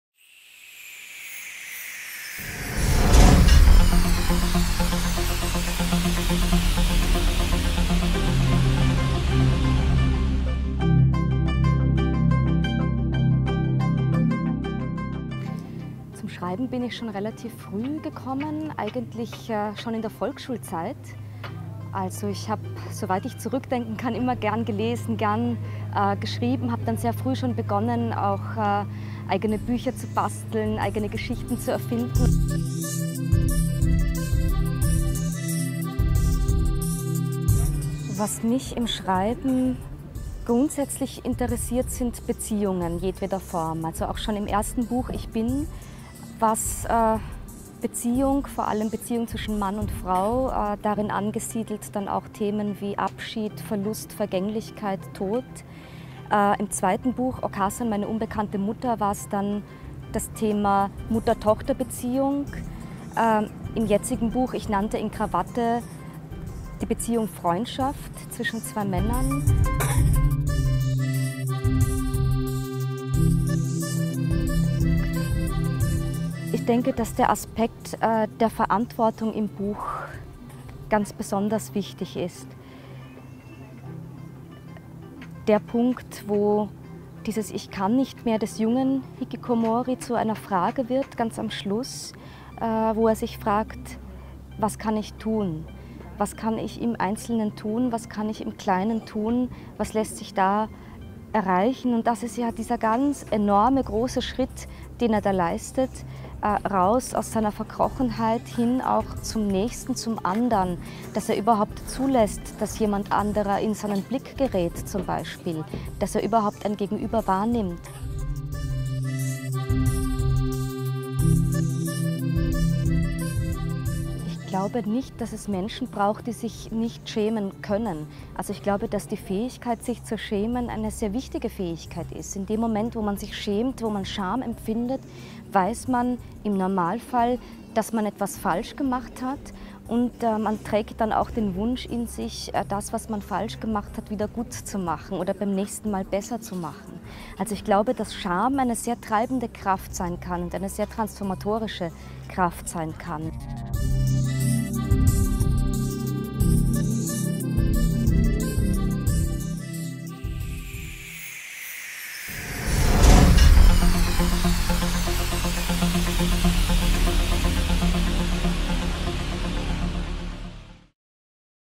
Literaturpreis Alpha 2012 - Gewinnerin Milena Michiko Flašar
Milena Michiko Flasar wurde für ihren Roman „Ich nannte ihn Krawatte" in die Alpha-Shortlist aufgenommen. Die in Wien lebende Tochter einer Japanerin und eines Österreichers nimmt darin Einblick in das Leben zweier Außenseiter, die sich auf einer Parkbank in Tokio begegnen. Flasars Roman ist von hoher Allgemeingültigkeit und Aktualität. Er stellt die Gefahr des Scheiterns an der Leistungsgesellschaft und die Einsamkeit in der Masse ebenso in den Fokus seiner Erzählung wie den Wert von Freundschaft und die Macht der Musik.

Das Buch erhalten Sie z.B. hier:
Buch der Siegerin 2012